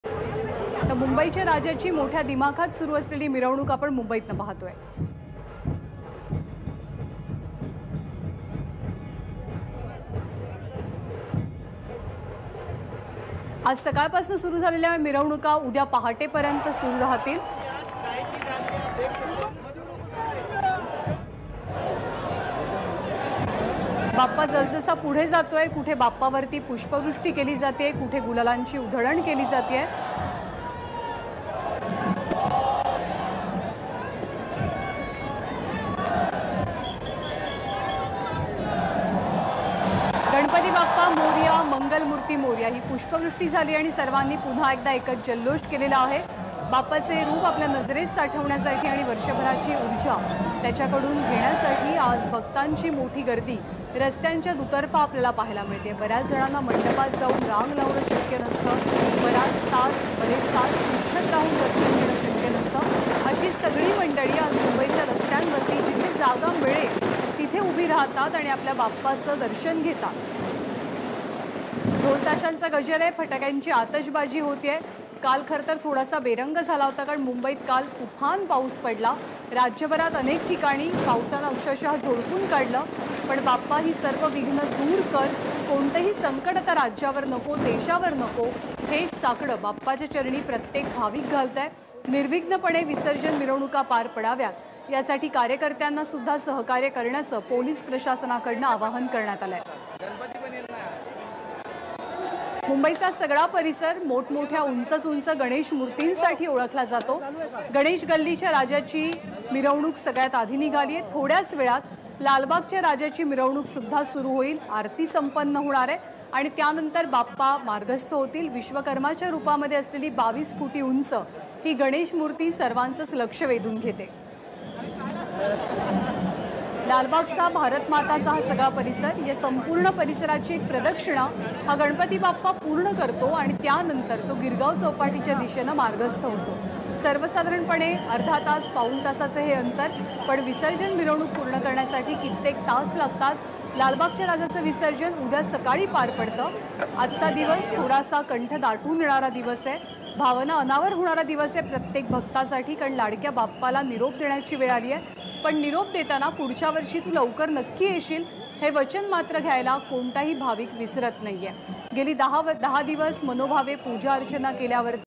Ganesh Galli Visarjan : मुंबईच्या राजावर पुष्पवृष्टी, गुलालासह मंगलमूर्ती मोरयाचा जयघोष
ABP Majha LIVE TV | Ganpati Visarjan | Yakub Memon | Queen Elizabeth | Marathi News | LIVE TV 24x7